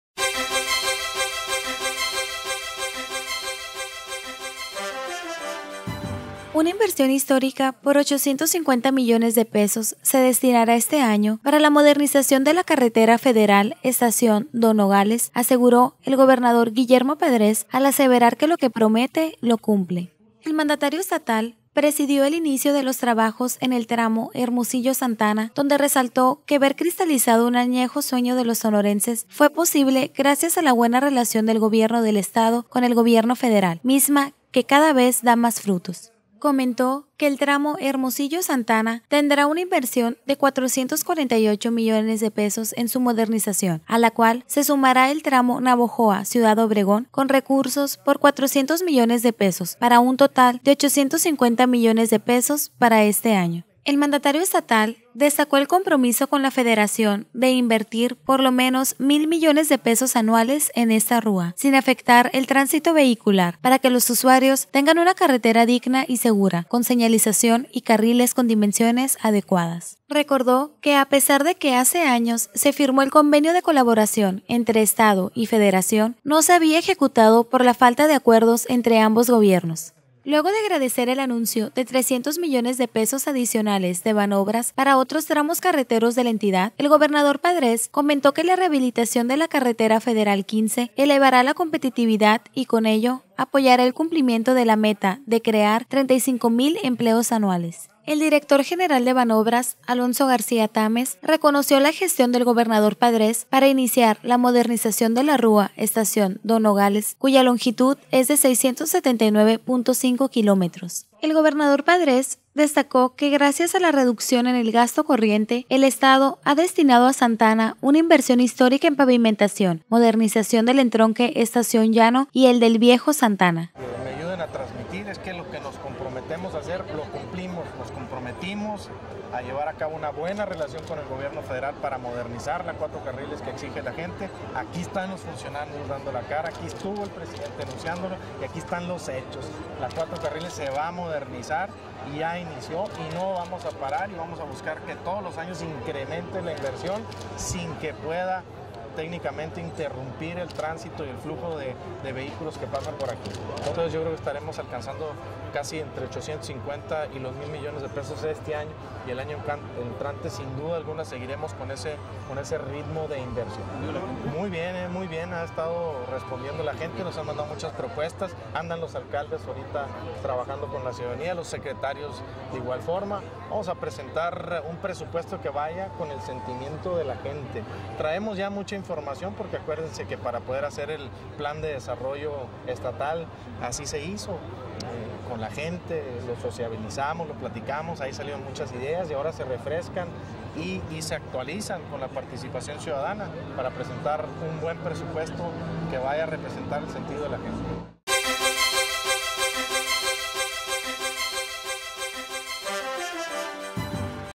Gobernador Guillermo Padrés presidió los trabajos en el tramo Hermosillo-Santa Ana. 09-11-2010
Santa Ana, Sonora.- Una inversión histórica por 850 millones de pesos se destinará este año, para la modernización de la carretera federal Estación Don-Nogales, aseguró el Gobernador Guillermo Padrés, al aseverar que lo que promete, lo cumple.